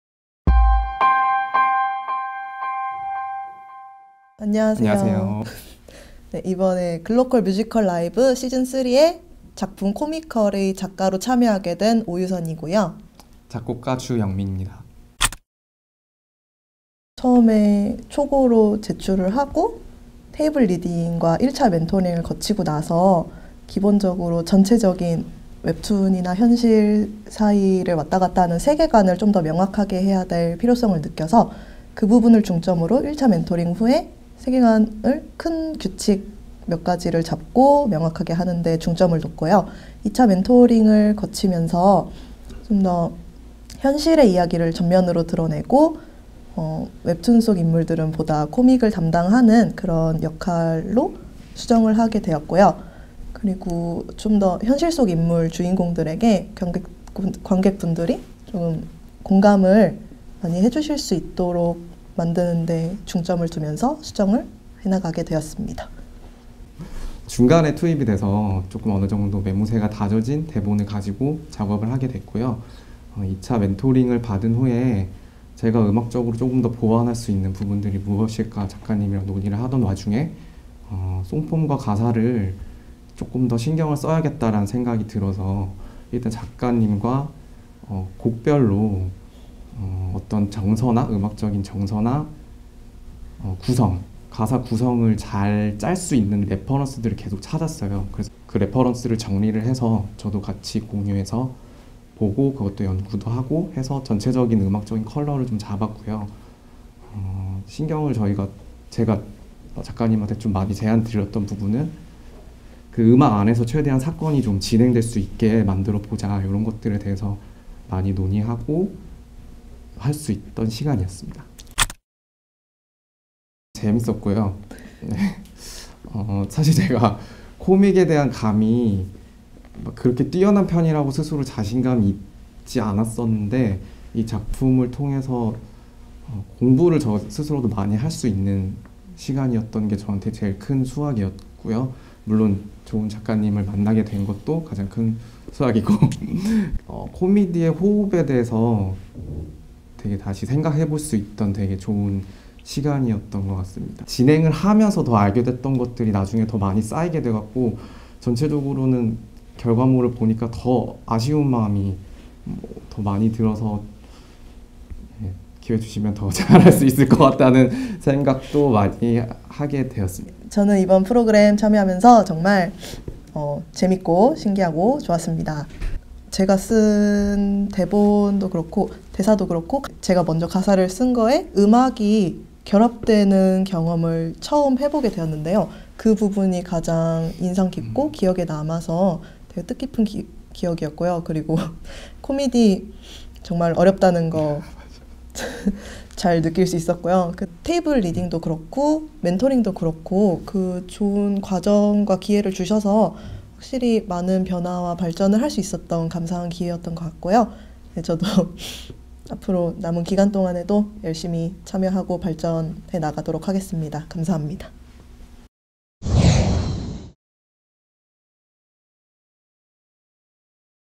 [글로컬 시즌3] 뮤지컬 '코믹컬' 창작자 인터뷰
글로컬 뮤지컬 라이브 시즌3
뮤지컬 '코믹컬' 창작자 인터뷰
오유선 작가, 주영민 작곡가